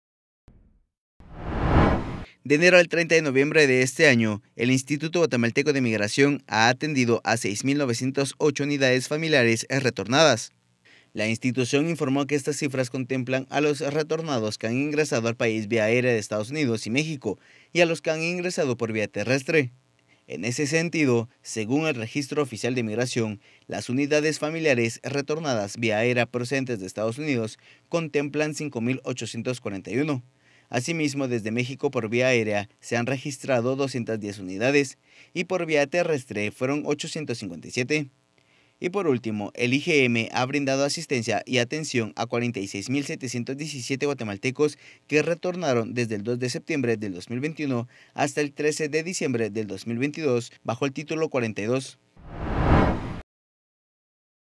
Por lo menos seis mil familias retoran durante 2022
Según Migración han atendido a más de seis mil familias han sido retornadas durante el 2022.